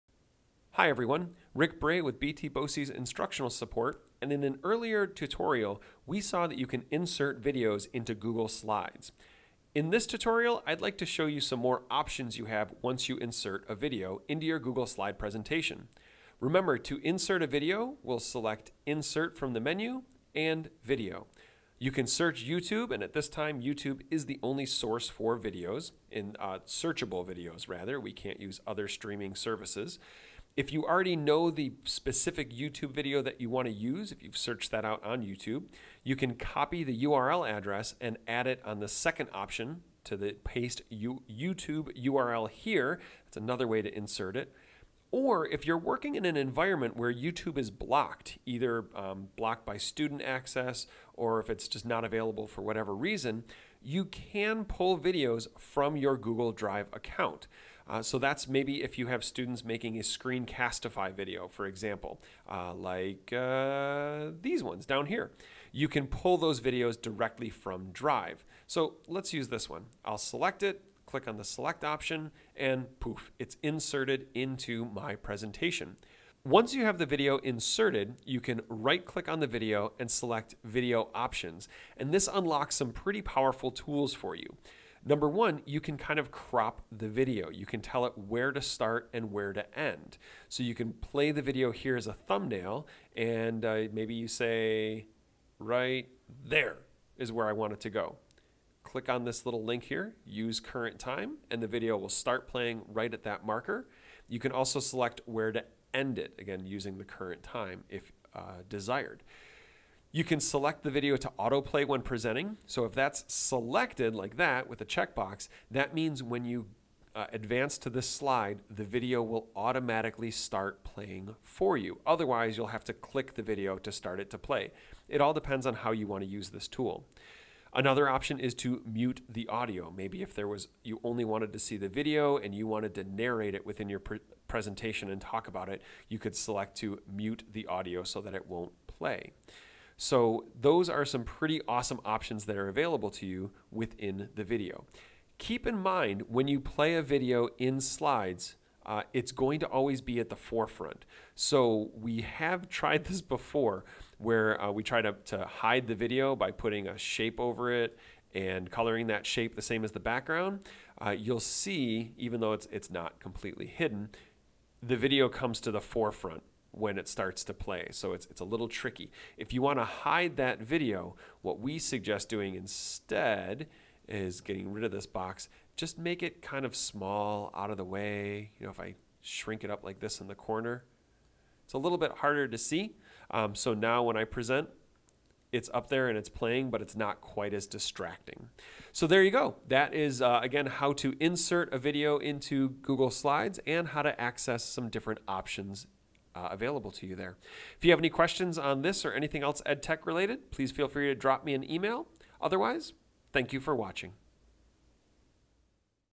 Google Slides: Video Options
This video show how to access video options such as cropping, autoplay, and mute audio for video files inserted into a Google Slide presentation.